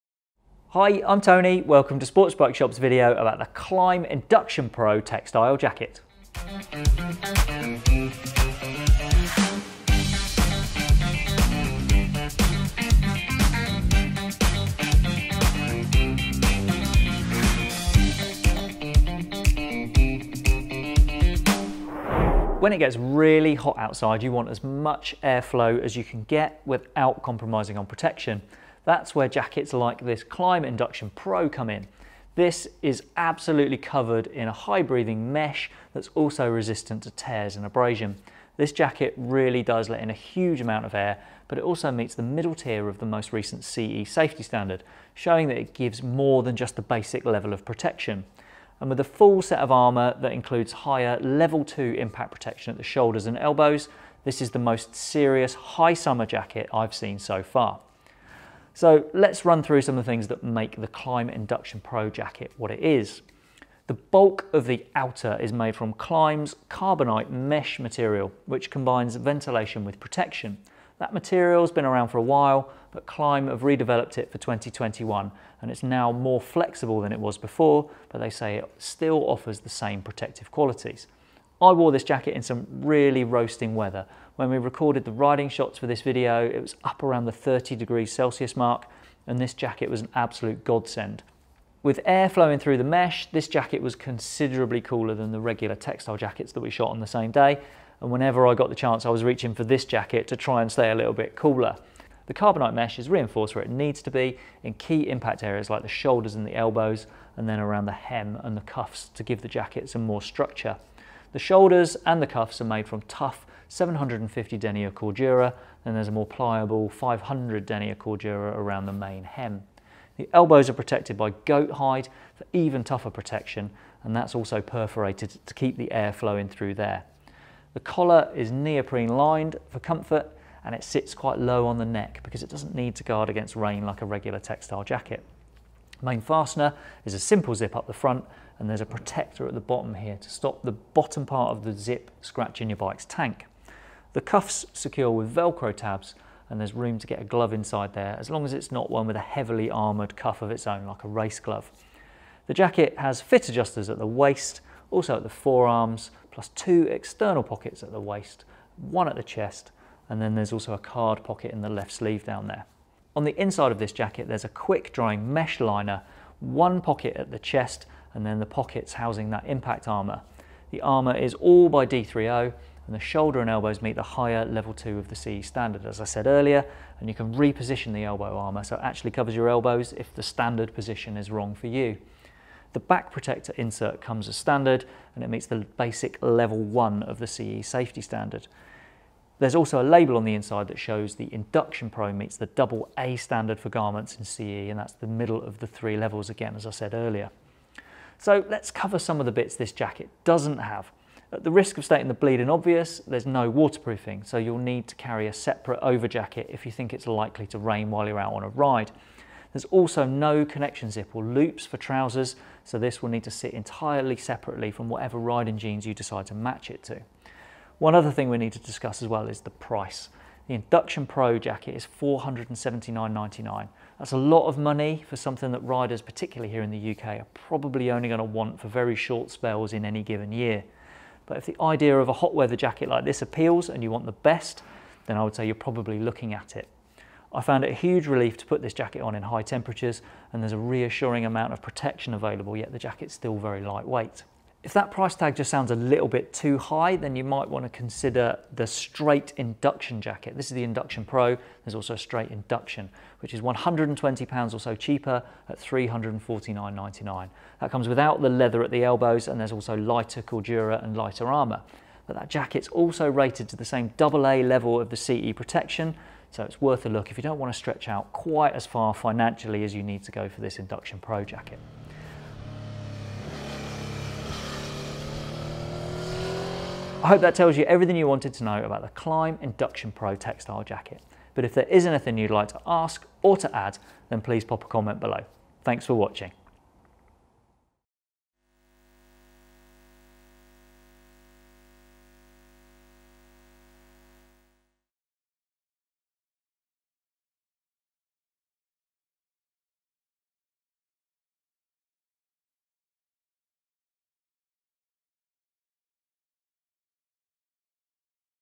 Klim Induction Pro motorcycle jacket review - Sportsbikeshop
Klim’s Induction Pro is quite possibly the best high-mesh textile bike jacket available. Find out all about it here.